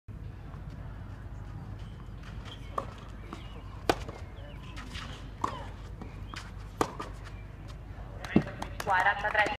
Caregaro vs Di Sarra - Day 5 Prequali
Martina Caregaro si guadagna l'accesso alla finale del torneo di prequalificazione femminile sconfiggendo per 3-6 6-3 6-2 Federica Di Sarra.